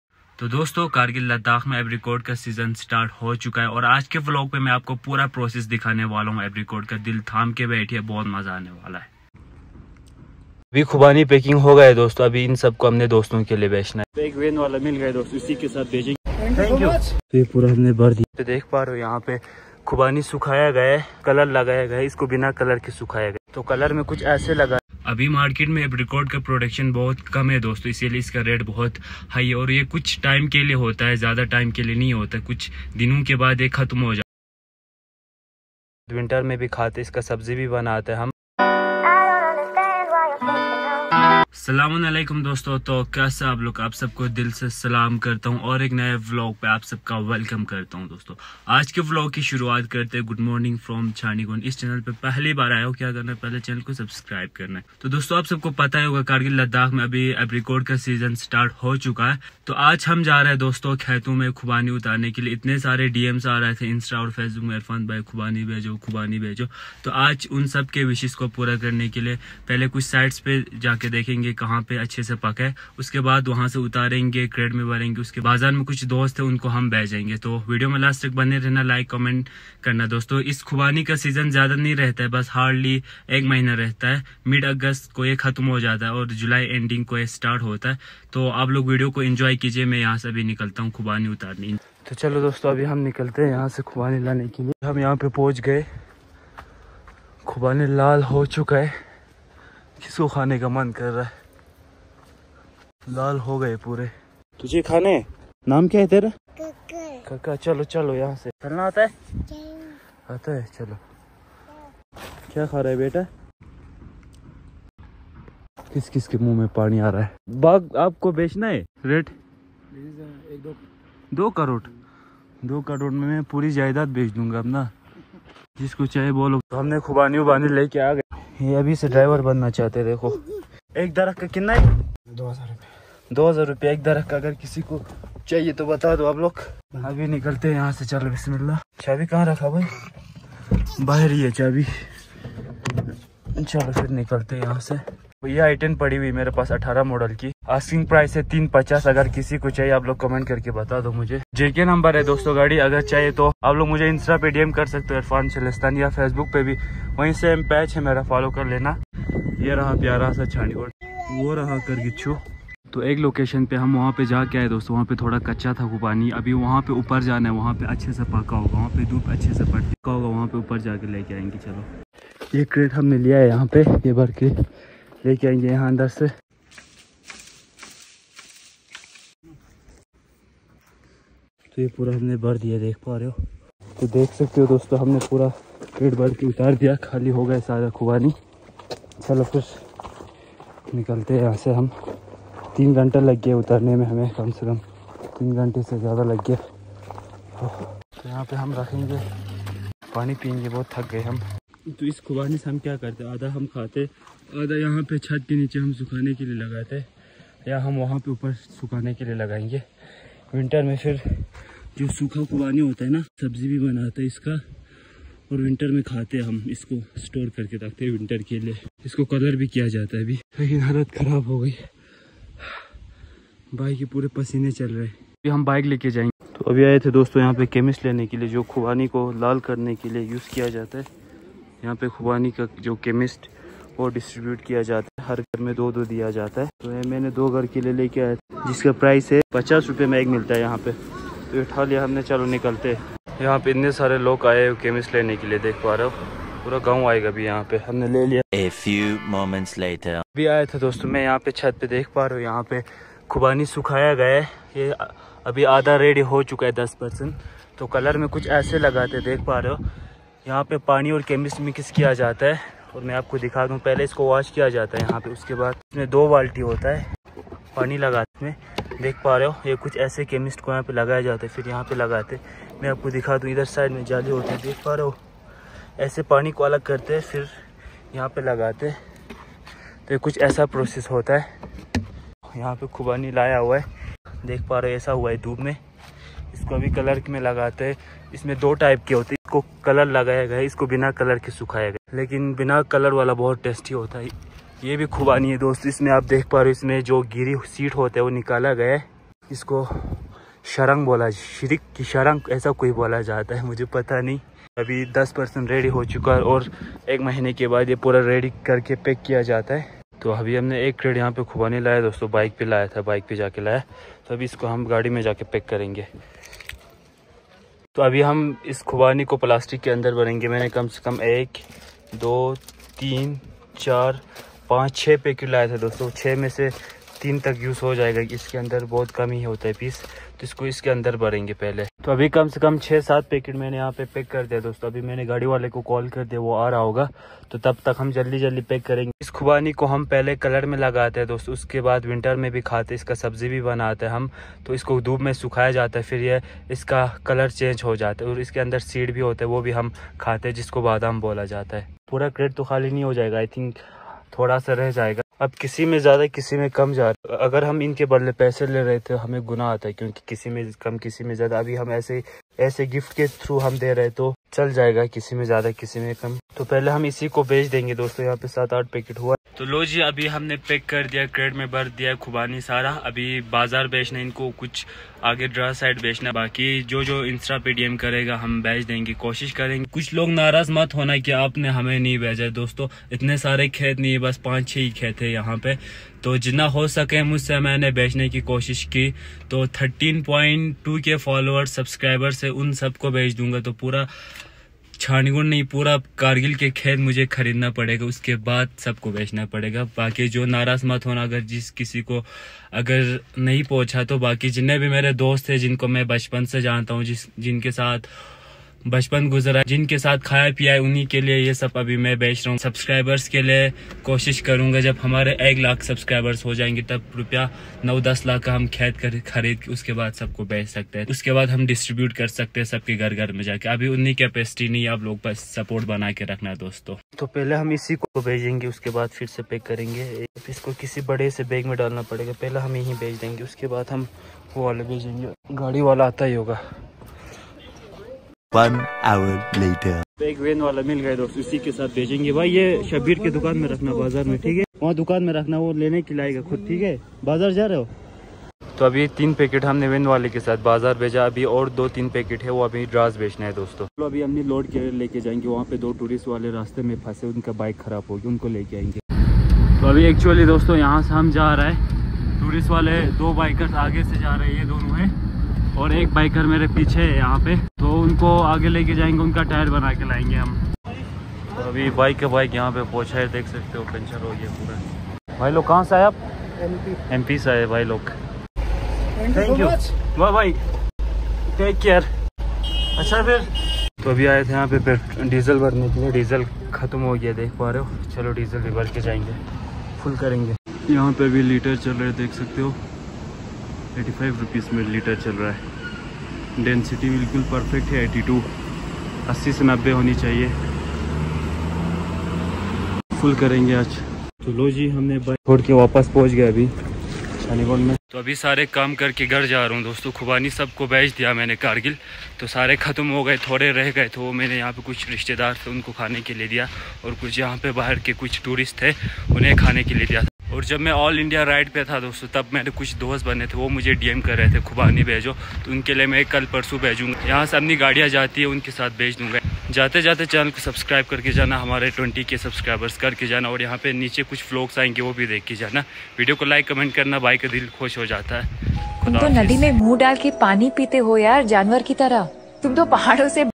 Subscribers ke Liya Apricot Gift kiyaa 😍
In this video i explore the village of Apricot

Apricot season in kargil
Apricot fruit
Apricot delusion fruit
Apricot in Himalayas
Apricot in Ladakh